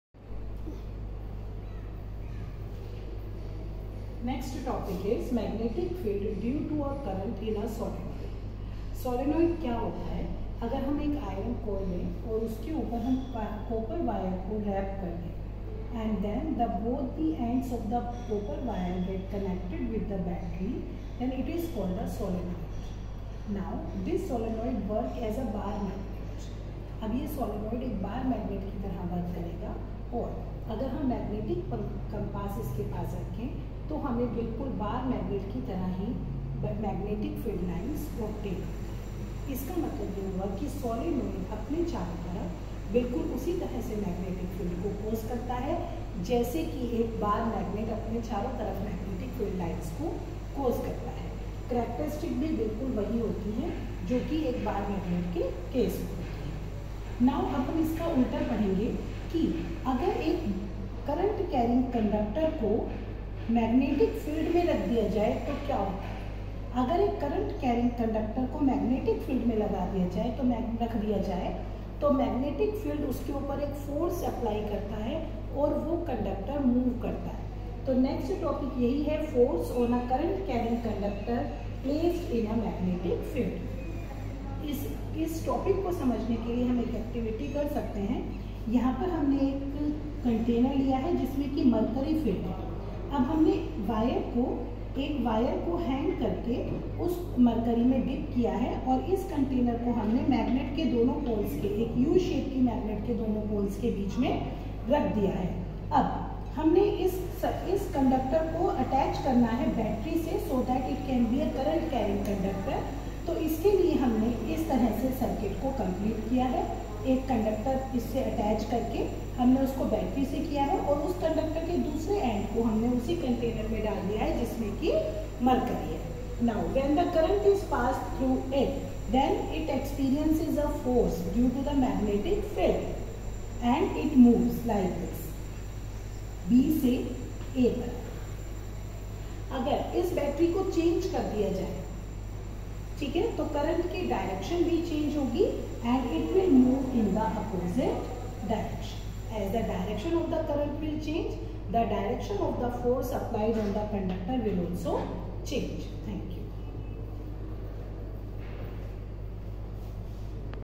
Class- X- Science (Physics)- Ch-12- MAGNETIC EFFECT OF ELECTRIC CURRENT- Part-3
Lectured by Mrs. Meenu Singhal (PGT), Maples Academy, Deoband, Distt.- Saharanpur (U.P)